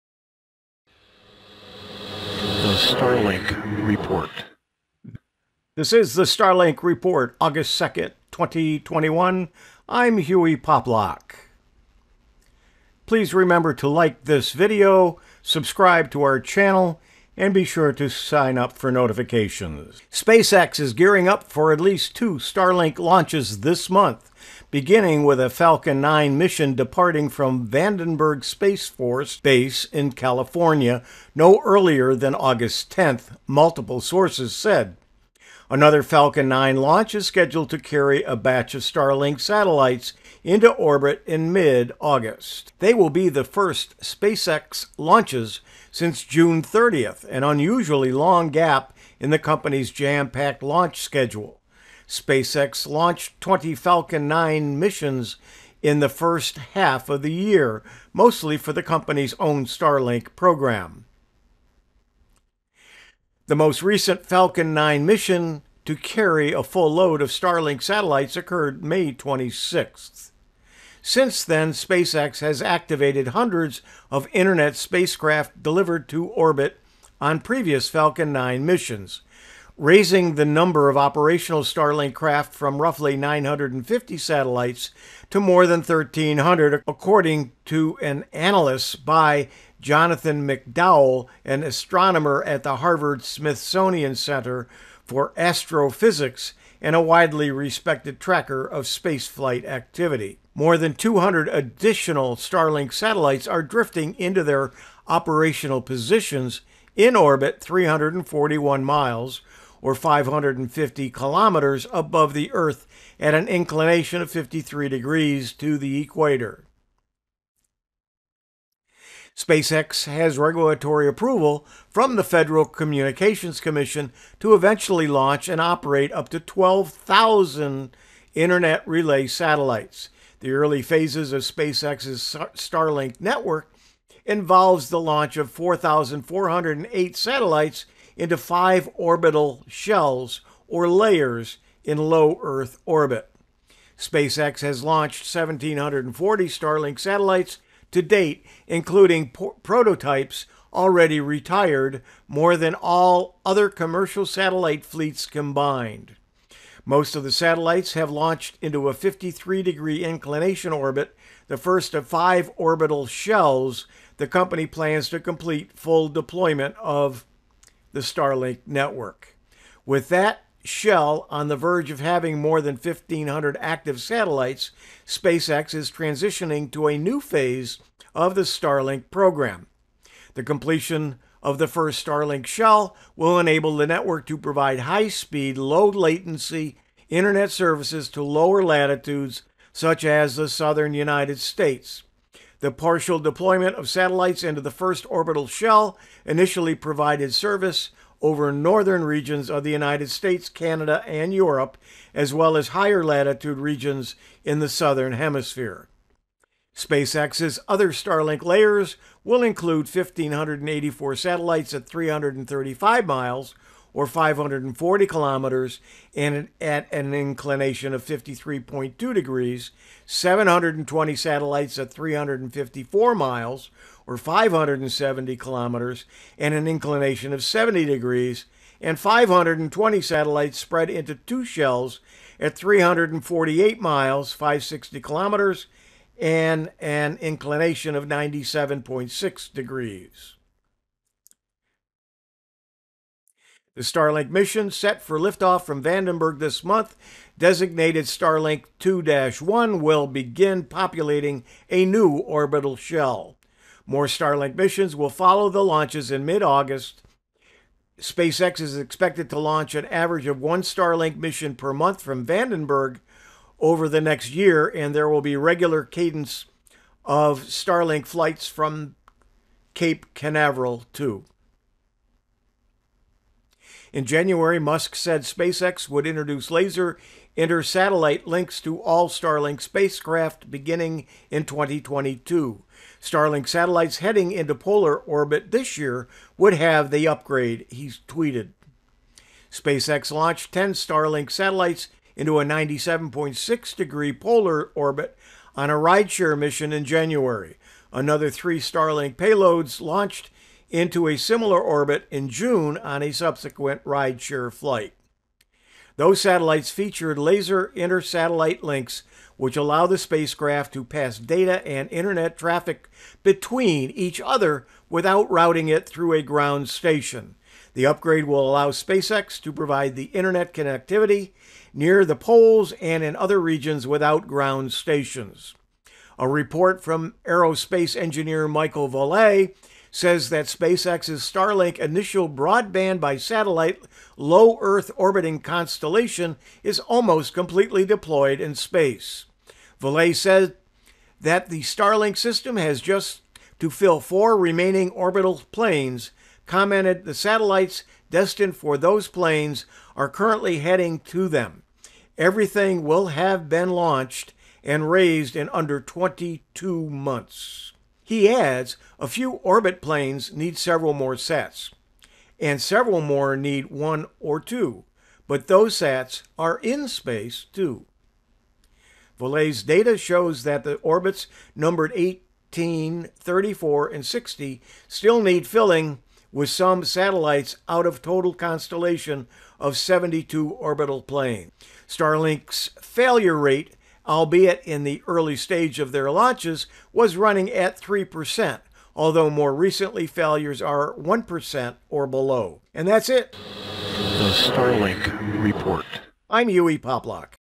The Starlink Report 2021 08 02
The Starlink Report for August 2, 2021. This report is the August 2021 episode of a series of reports about SpaceX's Starlink Satellite Internet Broadband service. It is part of the August 2 episode of Tech for Seniors which Zoom's every Monday and then is posted on YouTube.